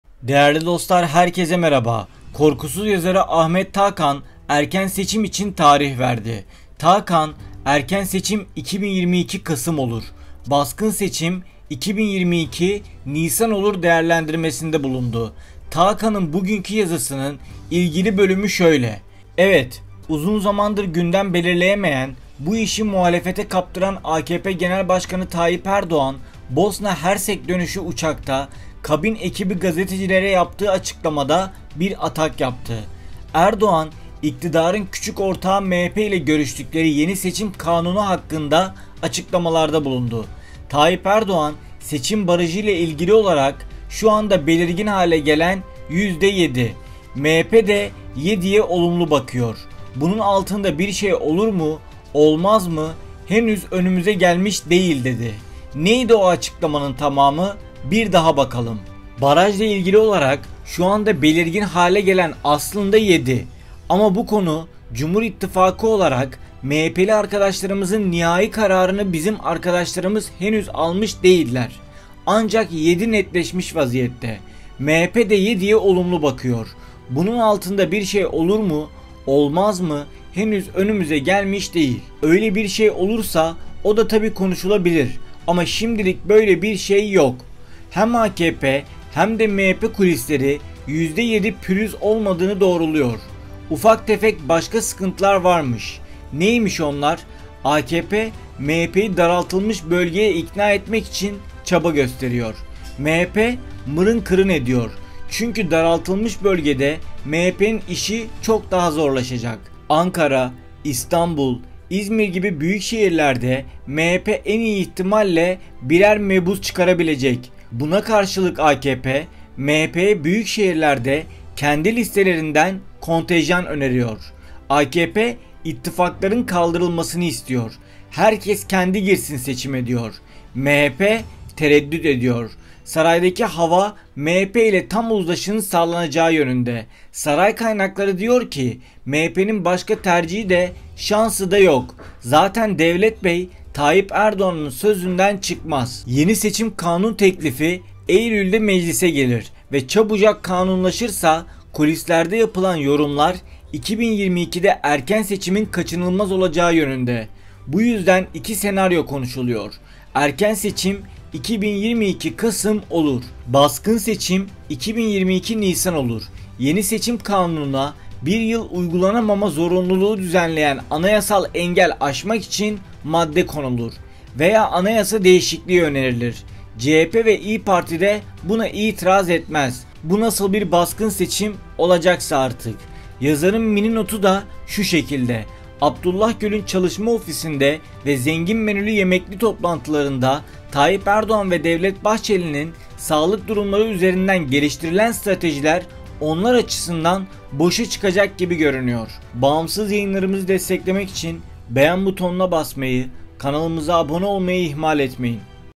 Erdoğan'ın aklındaki Seçim Tarihini Açıkladı
Korkusuz yazarı Ahmet Takan, erken seçim için tarih verdi. Takan, "Erken seçim 2022 Kasım olur. Baskın seçim 2022 Nisan olur." değerlendirmesini yaptı.

Takan'ın bugünkü yazısının bir bölümü şöyle:

Eveet!…

Uzun zamandır gündem belirleyemeyen, bu işi muhalefete kaptıran AKP Genel Başkanı Tayyip Erdoğan, Bosna Hersek dönüşü uçakta kabin ekibi gazetecilere yaptığı açıklamalarla bir atak yaptı. Erdoğan, iktidarın küçük ortağı MHP ile görüştükleri yeni seçim kanunu hakkında açıklamalarda bulundu. Tayyip Erdoğan, seçim barajıyla ilgili olarak “Şu anda belirgin hale gelen 7. MHP de 7’ye olumlu bakıyor. Bunun altında bir şey olur mu olmaz mı, henüz önümüze gelmiş değil” dedi. Neydi o açıklamanın tamamı?.. Bir daha bakalım;

“Barajla ilgili olarak şu anda belirgin hale gelen aslında 7 ama bu konuda Cumhur İttifakı olarak MHP’li arkadaşlarımızın nihai kararını bizim arkadaşlarımız henüz almış değiller. Ancak 7 netleşmiş vaziyette. MHP de 7’ye olumlu bakıyor. Bunun altında bir şey olur mu, olmaz mı, henüz önümüze gelmiş değil. Öyle bir şey olursa o da tabii konuşulabilir ama şimdilik böyle bir şey yok.”

Hem AKP hem de MHP kulisleri yüzde 7’de pürüz olmadığını doğruluyor. “Ufak tefek başka sıkıntılar var”mış… Neymiş onlar?

-AKP, MHP’yi daraltılmış bölgeye ikna etmek için çaba gösteriyor. MHP, mırın kırın ediyor. Çünkü, daraltılmış bölgede MHP’nin işi çok daha zorlaşacak. Ankara, İstanbul, İzmir gibi büyükşehirlerde MHP en iyi ihtimalle 1’er mebus çıkarabilecek. Buna karşılık AKP, MHP’ye büyükşehirlerde kendi listelerinden kontenjan öneriyor.

-AKP, ittifakların kaldırılmasını istiyor, “Herkes kendisi girsin seçime” diyor. MHP tereddüt ediyor.

Saray’daki hava MHP ile tam uzlaşının sağlanacağı yönünde… Saray kaynakları diyor ki; “MHP’nin başka tercihi de şansı da yok. Zaten Devlet bey, Tayyip Erdoğan’ın sözünden çıkmaz.”

Yeni seçim kanun teklifi eylülde Meclis’e gelir ve çabucak kanunlaşırsa, kulislerde yapılan yorumlar 2022’de erken seçimin kaçınılmaz olacağı yönünde. Bu yüzden 2 senaryo konuşuluyor;

-Erken seçim 2022 Kasım olur.

-Baskın seçim 2022 Nisan olur. Yeni seçim kanununa- 1 yıl uygulanamama zorunluğunu düzenleyen anayasal engel aşmak için- madde konulur. Veya anayasa değişikliği önerilir. CHP ve İYİ parti de buna itiraz edemez. (bu nasıl bir baskın seçim olacaksa -aht-)

Yazarın mini notu; Abdullah Gül’ün çalışma ofisinde ve zengin menülü yemekli toplantılarında Tayyip Erdoğan ve Devlet Bahçeli’nin sağlık durumları üzerinden geliştirilen stratejiler-onlar açısından- boşa çıkacak gibi görünüyor.